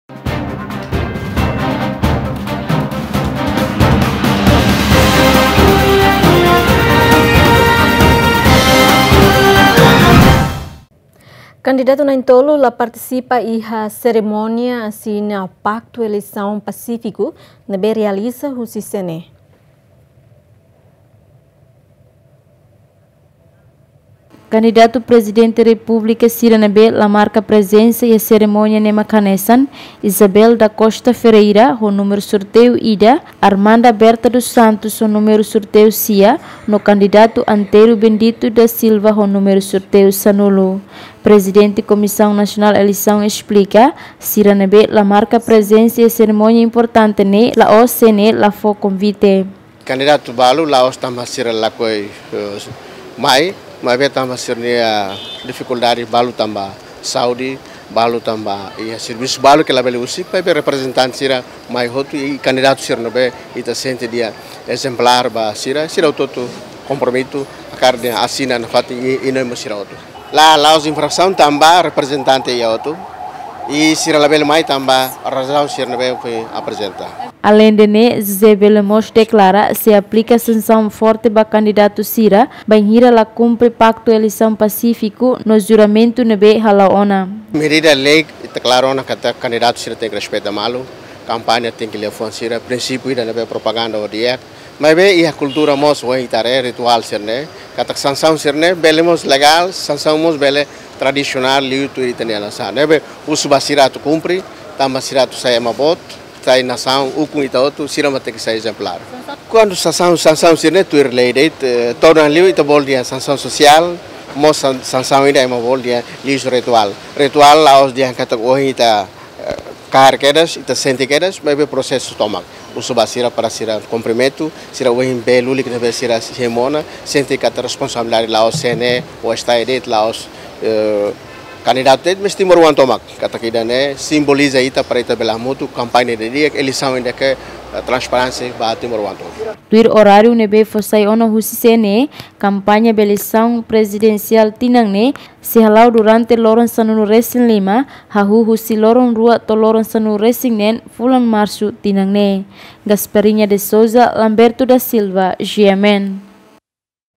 Kandidatu PR na’in tolu la partisipa iha asina PEP ho CNE
Kandidatu na'in tolu la partisipa iha seremonia asina Paktu Eleisaun Pasífiku ne’ebé realiza husi CNE